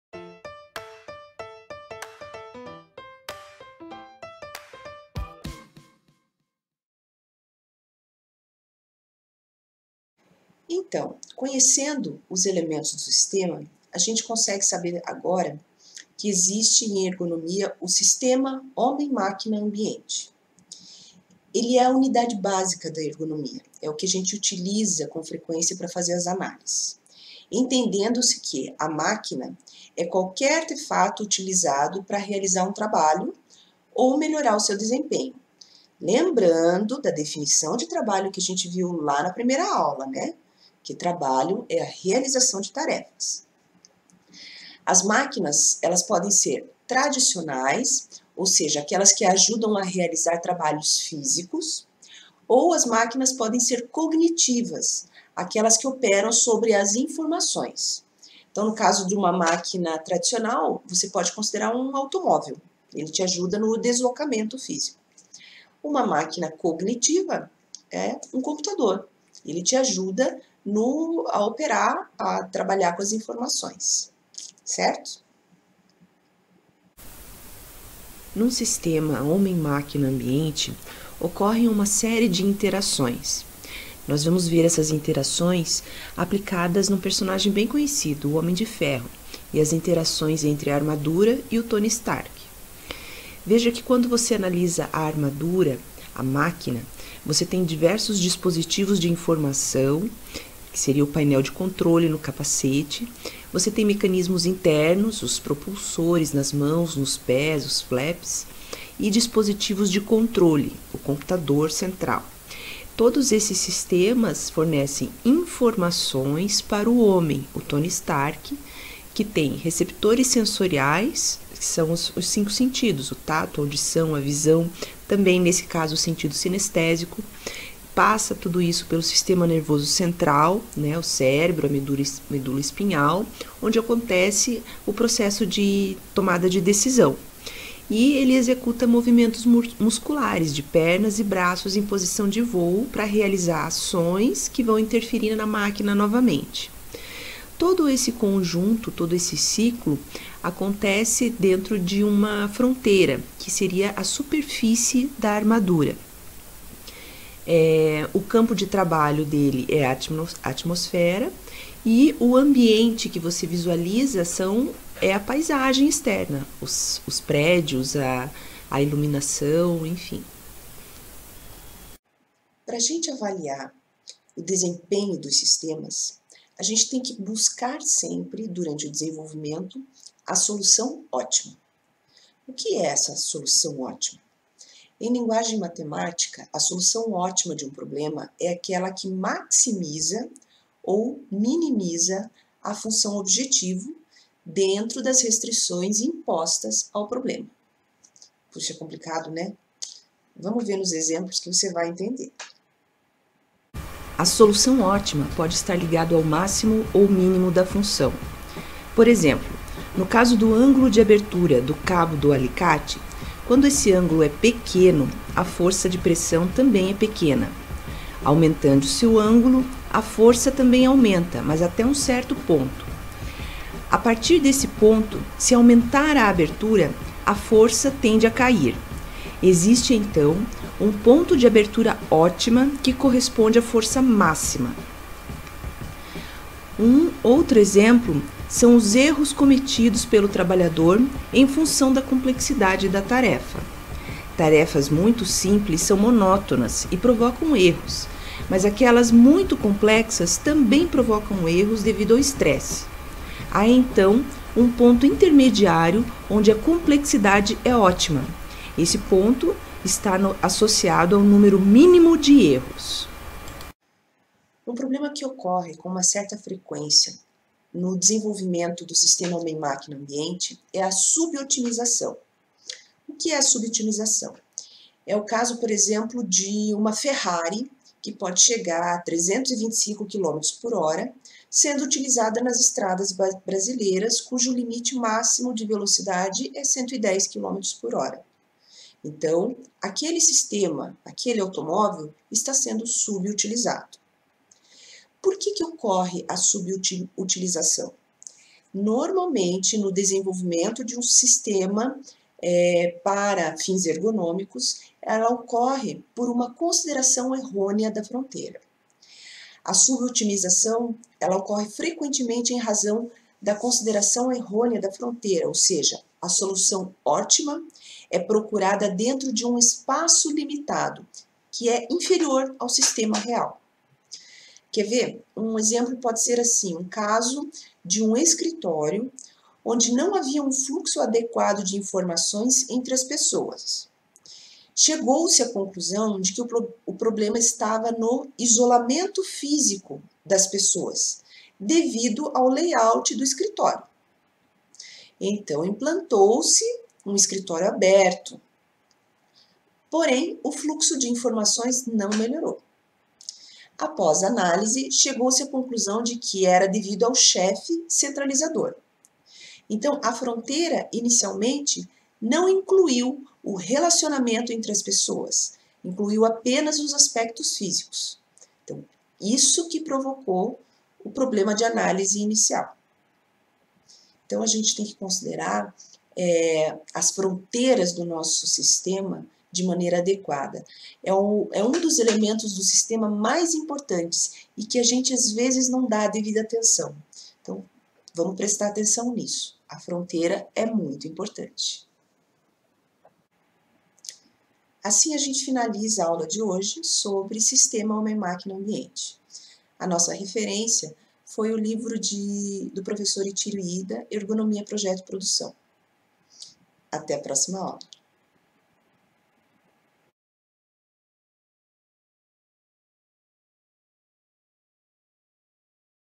Sistema HMA - parte 2
O vídeo apresenta as interações que ocorrem num sistema HMA, assim como os conceitos de solução ótima e de subutilização.

Referências:
IIDA, Itiro. Ergonomia: projeto e produção. 2ed. São Paulo: Blucher, 2005.